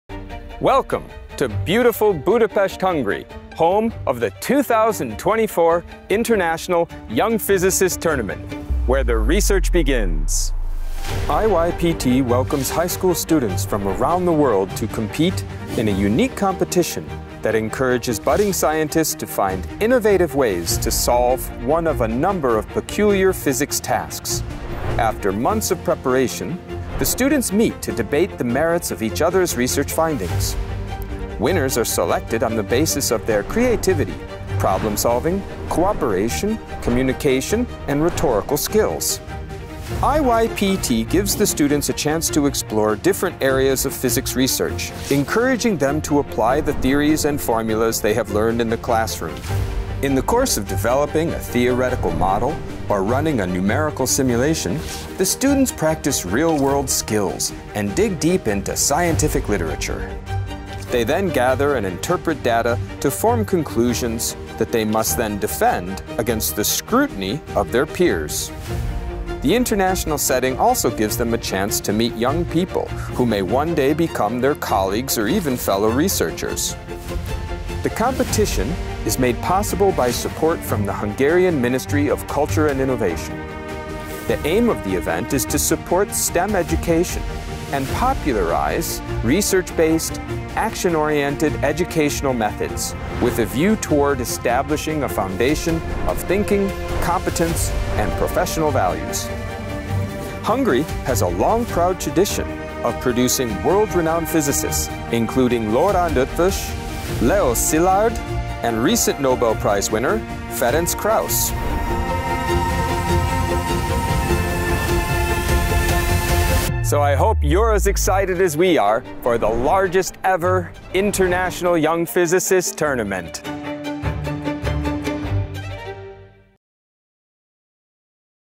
IYPT 2024 HUNGARY - where the research begins
What is IYPT?
The International Young Physicists’ Tournament (IYPT), sometimes referred to as the “Physics World Cup”, is a scientific competition between teams of secondary school students. It mimics, as close as possible, the real-world scientific research and the process of presenting and defending the results obtained.

Participants have almost a year to work on 17 open-ended inquiry problems. A good part of the problems involves easy-to-reproduce phenomena presenting unexpected behaviour.

The aim of the solutions is not to calculate or reach “the correct answer” as there is no such notion here. The Tournament is rather conclusions-oriented as participants have to design and perform experiments, and to draw conclusions argued from the experiments’ outcome.

The competition itself is not a pen-and-paper competition but an enactment of a scientific discussion (or a defence of a thesis) where participants take the roles of Reporter, Opponent and Reviewer and are evaluated by an international Jury.

The beauty of the Tournament is that teams can take quite different routes to tackle the same problem. As long as they stay within the broadly defined statement of the problem, all routes are legitimate and teams will be judged according to the depths reached by their investigations.

In 2013, IYPT was awarded the medal of the International Union of Pure and Applied Physics (IUPAP) in recognition of its inspiring and wide-ranging contribution to physics education that has touched many lives and countries, over the past 25 years.